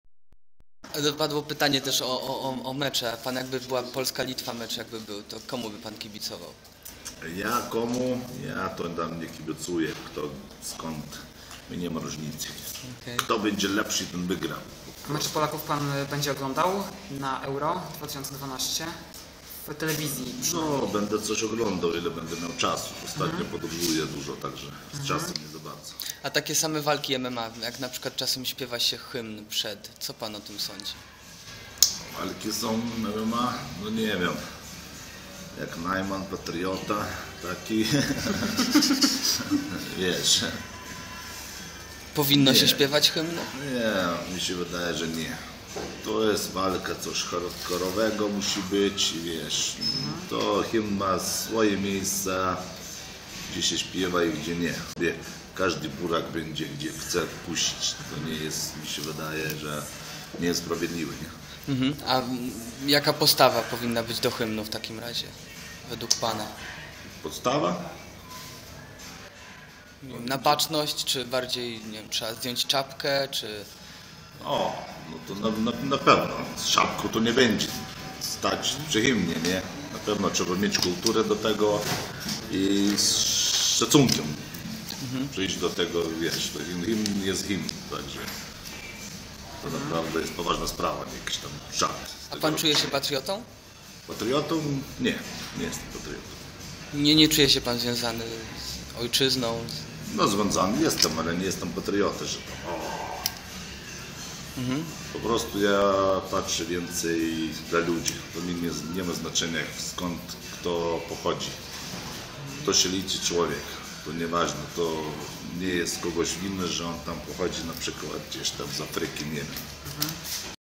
Hardkorowy Koksu o byciu patriotą i Marcinie Najmanie
Hardkorowy Koksu mówi nam o tym, czy czuje się patriotą i czy przed walkami MMA powinno się grać hymny. Jest też o Najmanie.